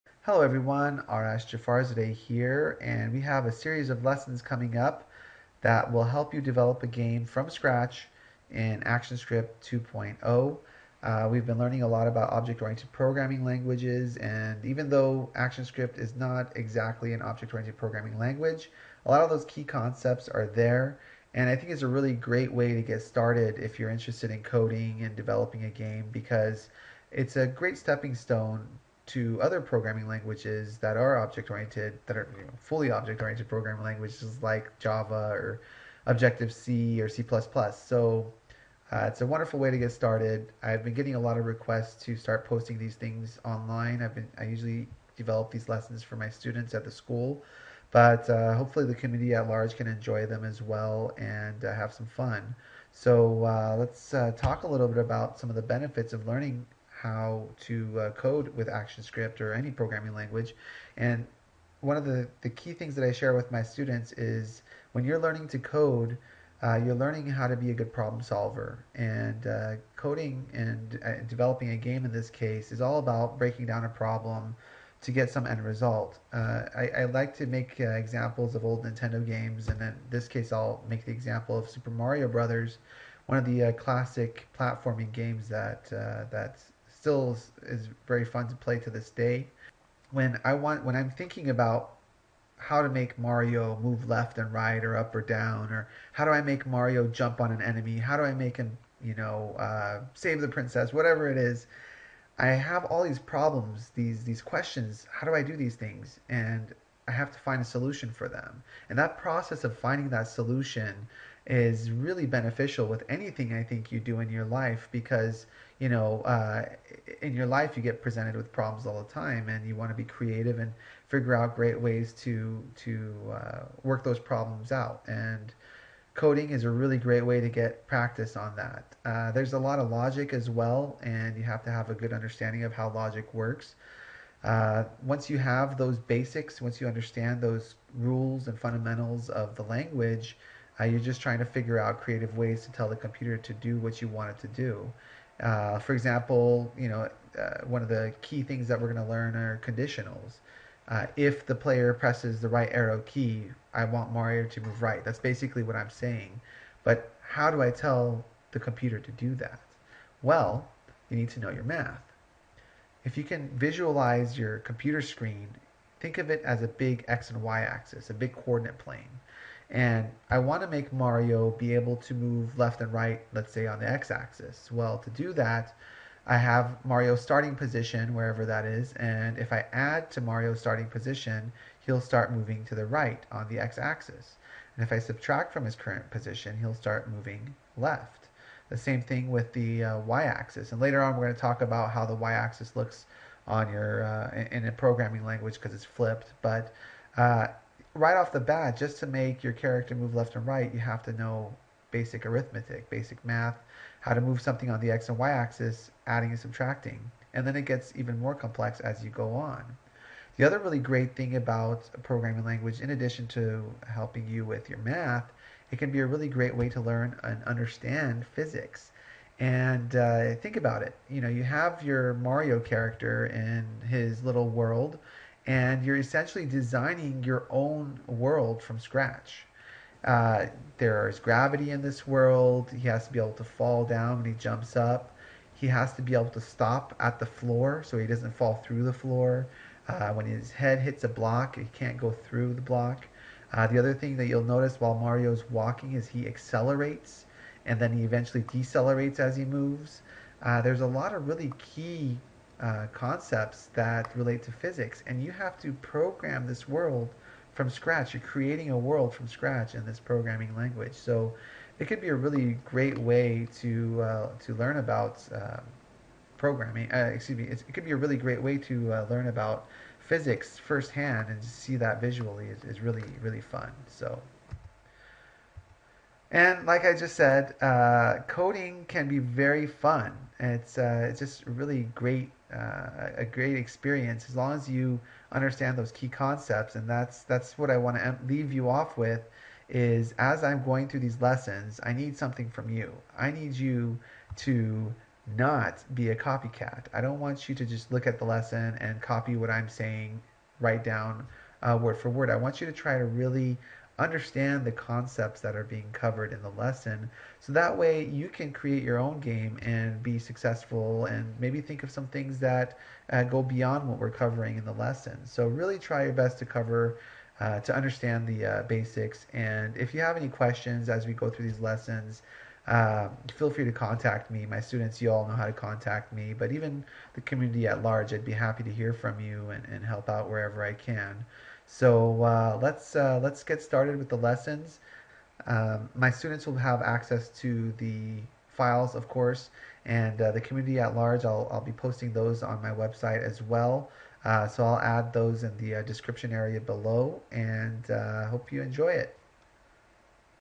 Develop a Game in ActionScript 2 - Intro
Time for a new series of lessons that will teach you how to develop your own game in Flash from start to finish!  This intro video will talk about what our goals are and some of the benefits to learning how to create your own game.